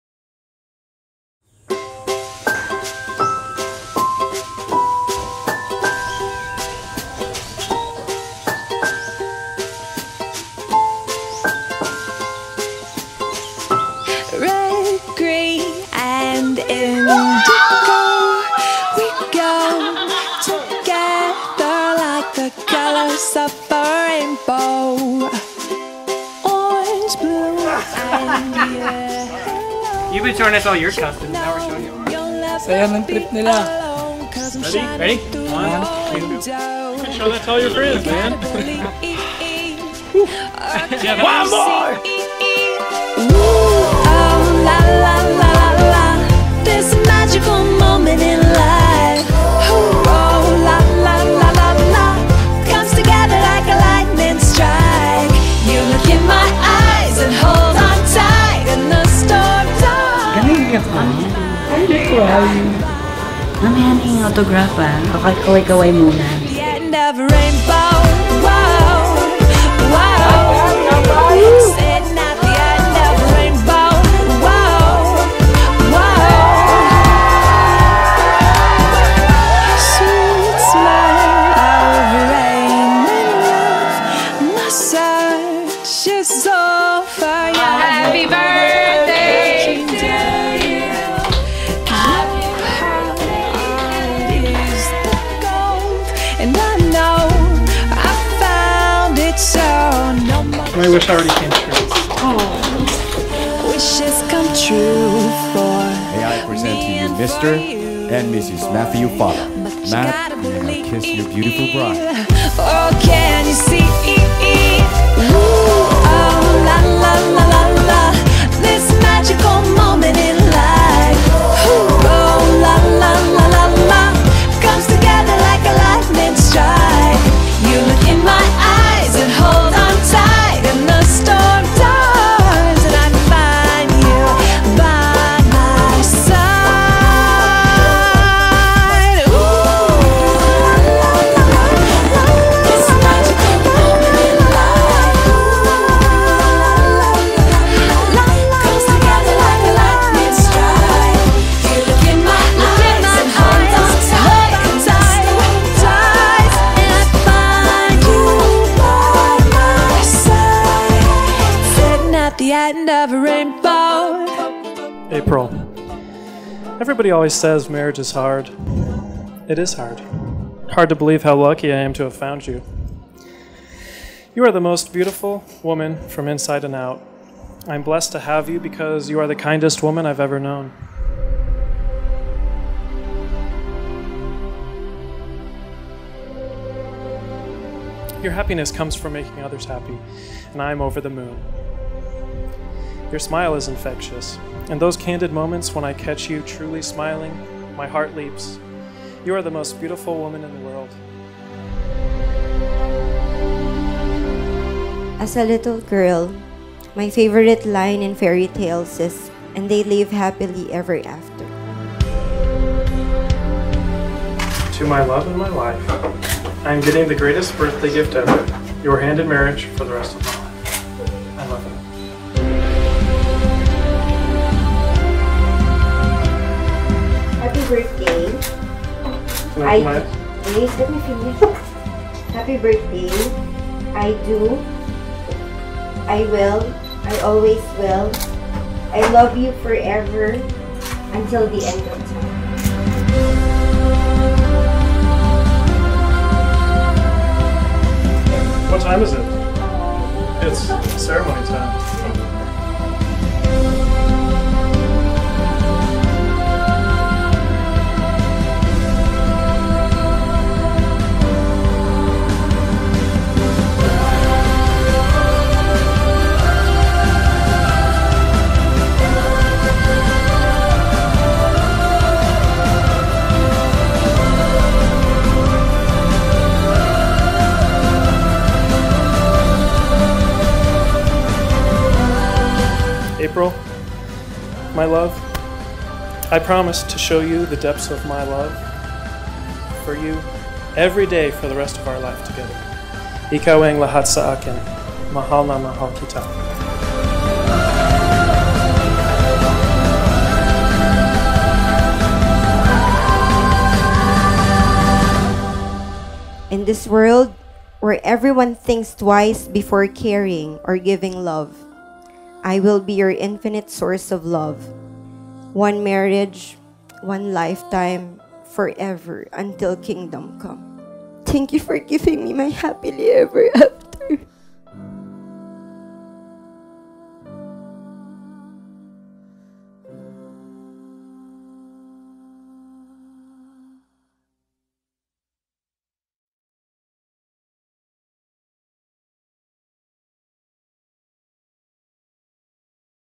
Mayad Boracay: Aprille and Matt "Infinity Contrived"
Everybody dreams of having their very own fairytale. Finding their own prince charming or damsel in distress to save. Their own special moment. Just like how Cinderella accidentally left her glass slipper while running down the staircase that led her to a happy ever after. But it's a totally different story for Aprille and Matt. Although there are no glass slippers involved but this time their love for each other is enough to bring their very own fairytale into a happily ever after.

"In this world where everyone thinks twice before caring or giving love, I will be your infinite source of love. One marriage, one lifetime forever until kingdom come. Thank you for giving me my happily ever after"